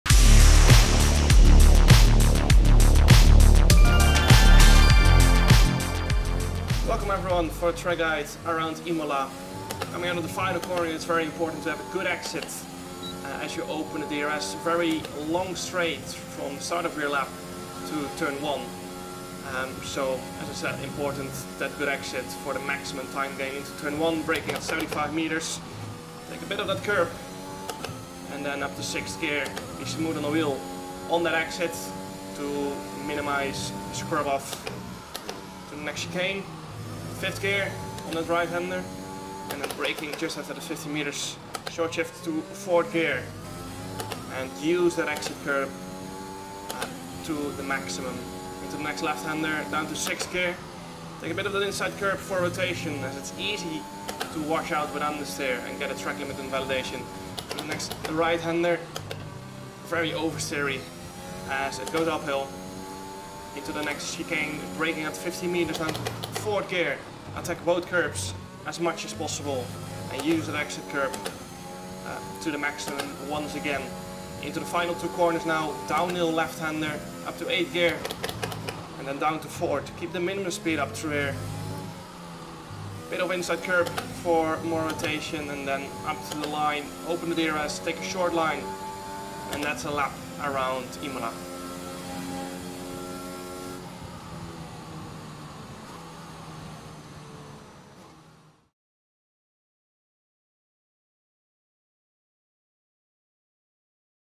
Taking on the Imola F1 Track with Jarno Opmeer! 🎮
Jarno's Track Guides are BACK 😜 Reigning F1 Esports champion @jarnoopmeer talks through how to master a hot lap of the Imola F1 track! 👀